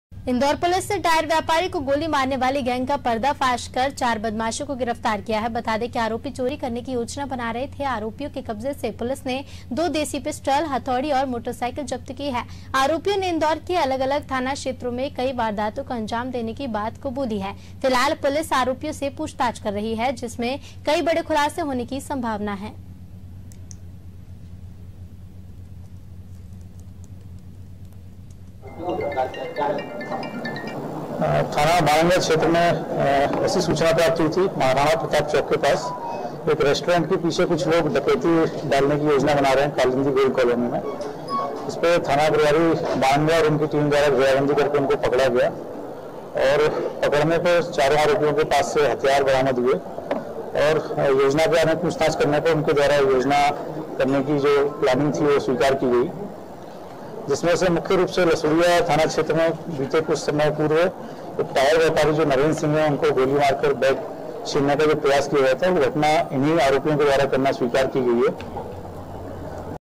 MP NEWS इंदौर : व्यापारी को गोली मारने का मामला | Aaj ki Taza Khabar | Anaadi Tv | Hindi News
इंदौर पुलिस ने टायर व्यापारी को गोली मारने वाली गैंग का पर्दाफाश करते हुए चार बदमाशों को गिरफ्तार किया है...बता दे आरोपी चोरी करने की योजना बना रहे थे...आरोपियों के कब्जे से पुलिस ने दो देशी पिस्टल, हथोड़ी और मोटसाइकिल जब्त की है... आरोपियों ने इंदौर के अलग अलग थाना क्षेत्रों में कई वारदातों को अंजाम देने की बात कबूली है...फिलहाल पुलिस आरोपियों से पूछताछ कर रही है...जिसमें कई बड़े खुलासे होने की संभावना है.